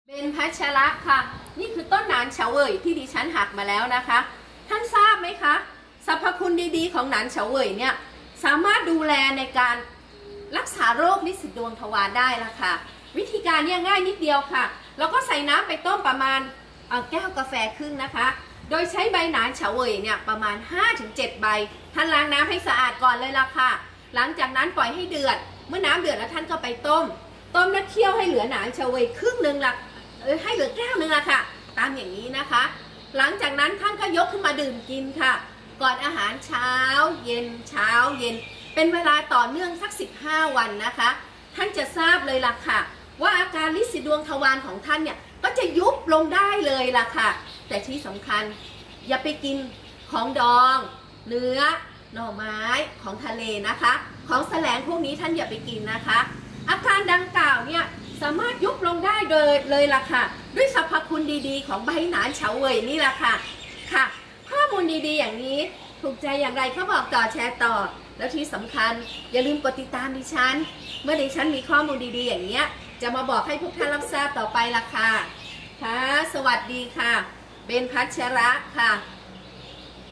480. ริดสีดวงทวาร ยุบ ยุบ ฝ่อลงเลยย
ริดสีดวงทวาร ยุบ ยุบ ฝ่อลงเลยย ห้ามกืน ของดอง เนื้อ หน่อไม้ ของทะเล